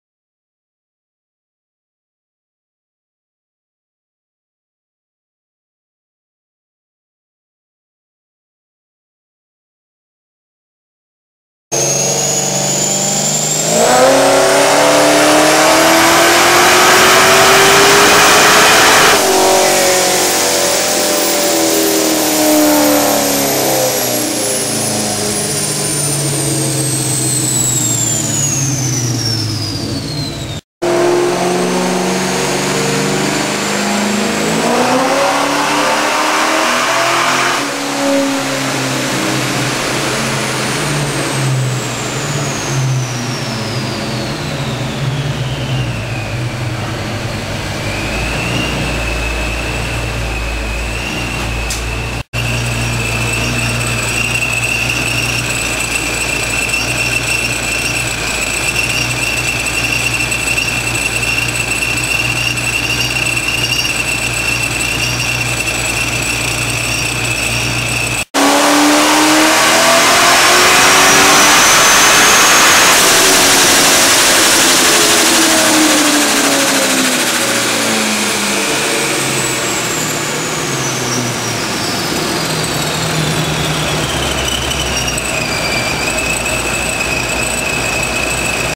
Jay's ProCharged Monte SS on Dyno
86 Monte SS, SDCE built blower engine, ProCharger P1SC, SDCE prepped carb with SDCE Fuel Tool (R).
Engine was engine-dynoed producing 670FWHP/586 lb-ft, car was chassis dyno'd producing 544RWHP/490 lb-ft on 10PSI of boost.  With the old engine and blower kit Jay drove to the track in NH from RI, ran an 11.1 @ 121mph on pump gas weighing in at 3920 #'s, got thrown out for not having a cage, and drove home! Watch for more vids of Jay's car coming soon!